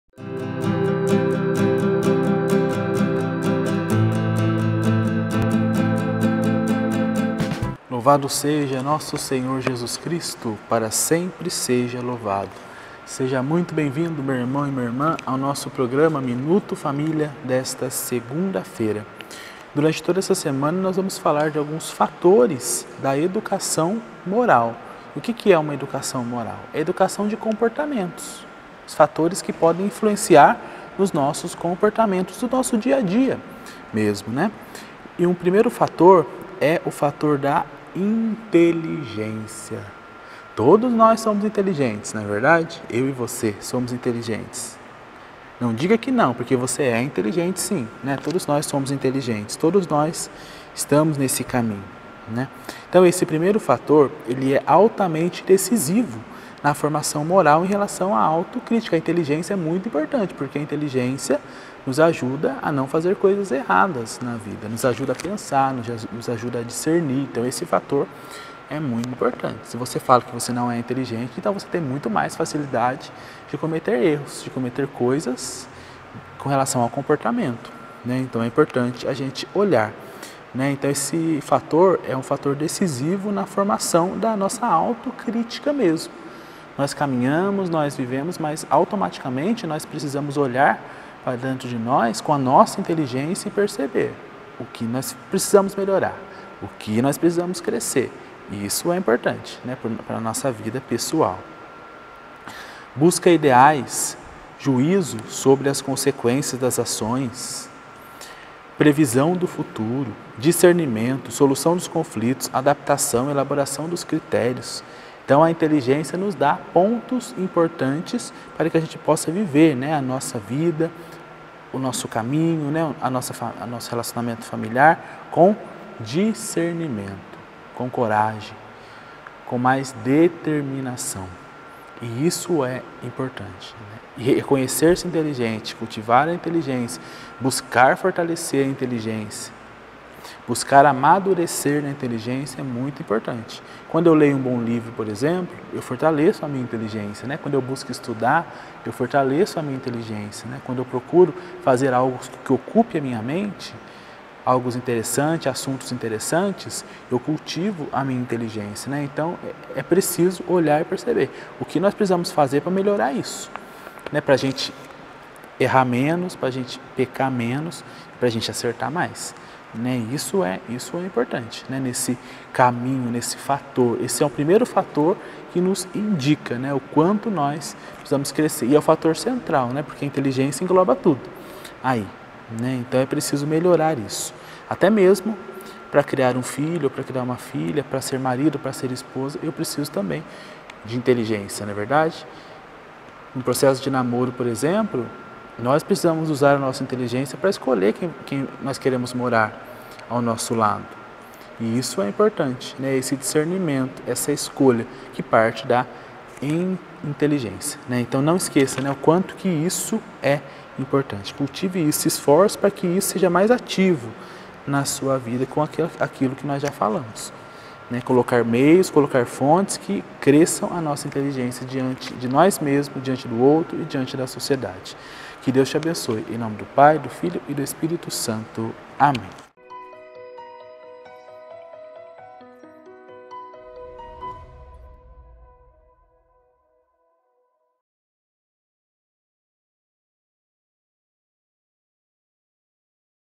MINUTO FAMÍLIA - OS FATORES DA EDUCAÇÃO - INTELIGÊNCIA - SANTUÁRIO FREI GALVÃO
Precisamos pedir ao Senhor Deus a inteligência para solucionar os conflitos familiares e buscar a educação dos filhos.